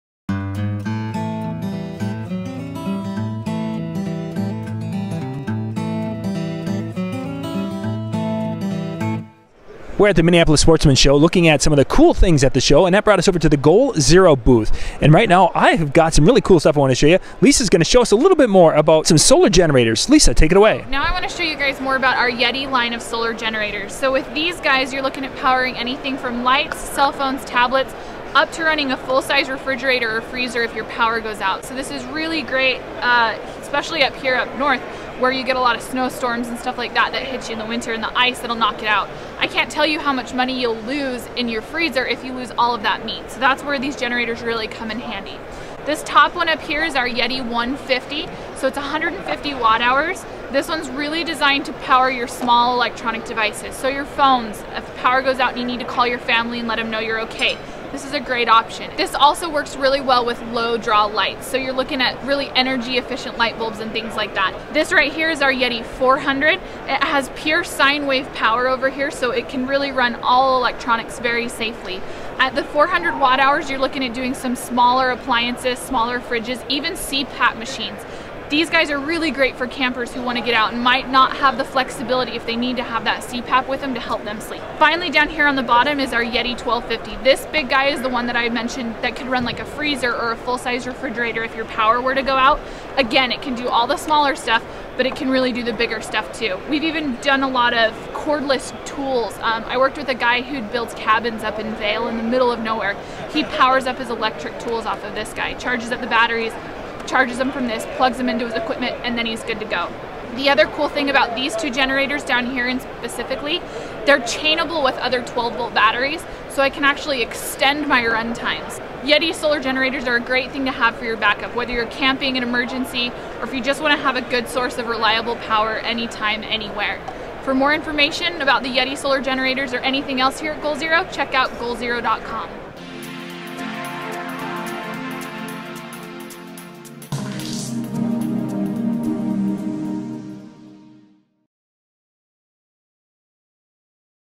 Yeti Solar Generators from GoalZero: By John Young of the Weekend Handyman
Recharge by: AC, 12V, Solar
Pwr Output: USB, 12V, AC, Anderson Power Pole
Ideal for: Laptop, Mini Fridge, TV, Backup, Emergency, Lights
Capacity: 1200Wh, 100Ah
Weight: 103 lbs (46.7 kg)
Chainable: Yes
The epitome of unlimited, portable backup power for complete peace of mind. From phones to refrigerators, and everything in between, with the Goal Zero Yeti 1250 Solar Generator, you have the power to back up your life in any situation. Ditch the noise, fumes and gasoline and enjoy unlimited power from the wall or the sun.